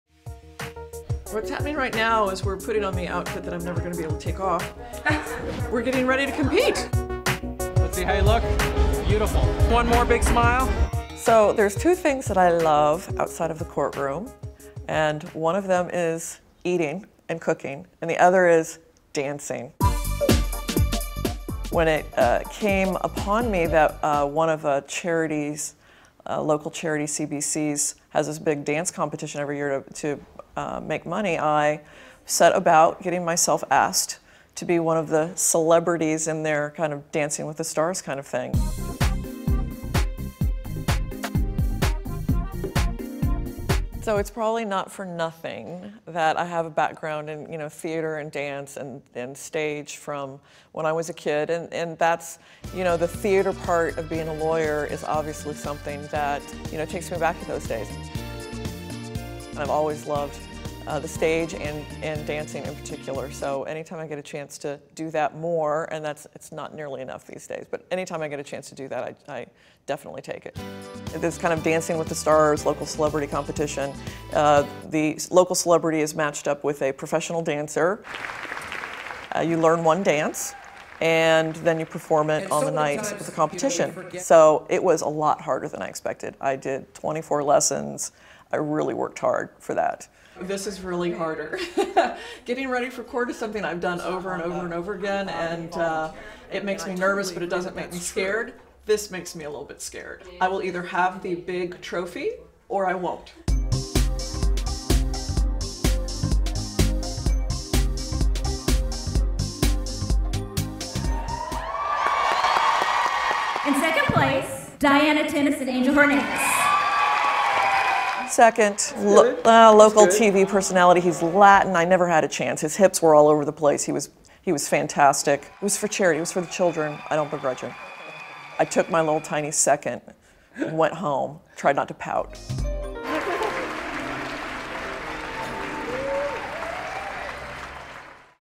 Dancing with former defense attorney Diana Tennis
Caryn Kelley's former defense attorney talks about what she loves to do outside of the courtroom and gives a glimpse into her background in dance and theatre.